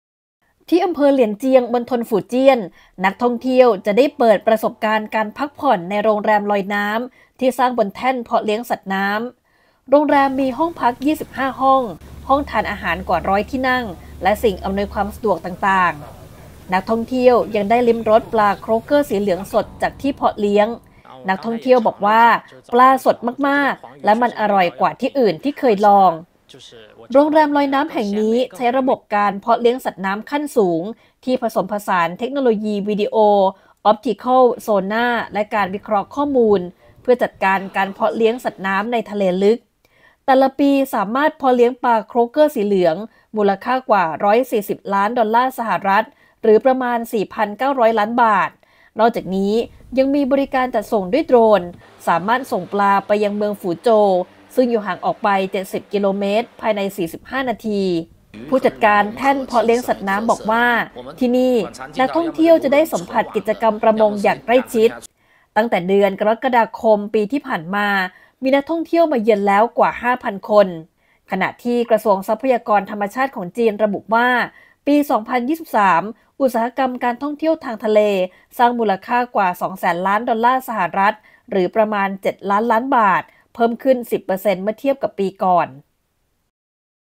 มณฑลฝูเจี้ยนสร้างโรงแรมบนแท่นเพาะเลี้ยงสัตว์น้ำ เปิดประสบการณ์การท่องเที่ยวรูปแบบใหม่
ที่อำเภอเหลียนเจียง มณฑลฝูเจี้ยน นักท่องเที่ยวจะได้เปิดประสบการณ์การพักผ่อนในโรงแรมลอยน้ำ ที่สร้างบนแท่นเพาะเลี้ยงสัตว์น้ำ โรงแรมมีห้องพัก 25 ห้อง ห้องรับประทานอาหารกว่า 100 ที่นั่ง และสิ่งอํานวยความสะดวกต่างๆ
นักท่องเที่ยวยังได้ลิ้มรสปลาโครกเกอร์สีเหลืองสดจากที่เพาะเลี้ยง Xie Fei นักท่องเที่ยวบอกว่า ปลาสดมากๆ มันอร่อยกว่าที่อื่นที่เคยลอง
โรงแรมลอยน้ำแห่งนี้ใช้ระบบการเพาะเลี้ยงสัตว์น้ำขั้นสูง ที่ผสมผสานเทคโนโลยีวีดีโอ ออปติคัล โซนาร์ และการวิเคราะห์ข้อมูล เพื่อจัดการการเพาะเลี้ยงสัตว์น้ำในทะเลลึก แต่ละปีสามารถเพาะเลี้ยงปลาโครกเกอร์สีเหลืองมูลค่ากว่า 140 ล้านดอลลาร์สหรัฐ หรือประมาณ 4,900 ล้านบาท นอกจากนี้ ยังมีบริการจัดส่งด้วย โดรน สามารถส่งปลาไปยังเมืองฝูโจว ซึ่งอยู่ห่างออกไป 70 กิโลเมตร ภายใน 45 นาที
Xu Huang ผู้จัดการแท่นเพาะเลี้ยงสัตว์น้ำ กล่าวว่า ที่นี่นักท่องเที่ยวจะได้สัมผัสกิจกรรมประมงอย่างใกล้ชิด ตั้งแต่เดือนกรกฎาคม 2023 มีนักท่องเที่ยวมาเยือนกว่า 5,000 คน
ขณะที่ กระทรวงทรัพยากรธรรมชาติของจีนระบุว่า ในปี 2023 อุตสาหกรรมการท่องเที่ยวทางทะเล สร้างมูลค่ากว่า 200,000 ล้านดอลลาร์สหรัฐ หรือประมาณ 7 ล้านล้านบาท เพิ่มขึ้น 10% เมื่อเทียบกับปีก่อน